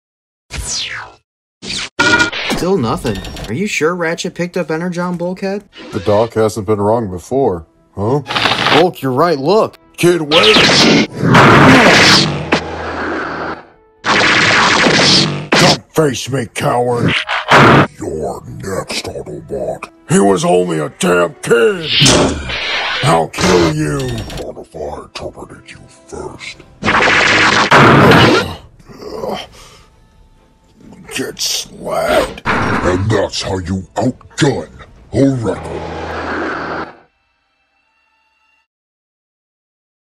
Skyquake Strikes! (Transformers Stop-Motion Short)
Skyquake runs into Bulkhead and Cloud-Breaker, he makes sure to make quick work out of them!

Transformers Legacy Evolution Leader Skyquake

Transformers Stop-Motion Shorts

Megatronic Studios